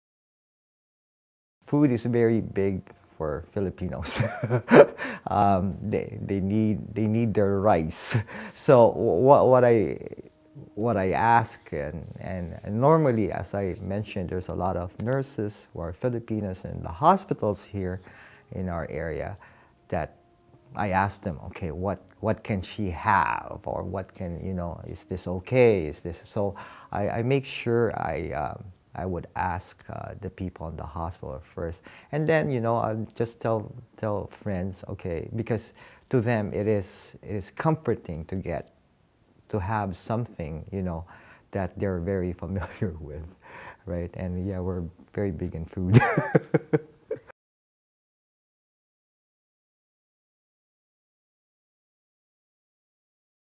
Jonathan: Providing comfort with food
In this video by Canadian Virtual Hospice, Jonathan speaks about the importance of food in the Filipino culture.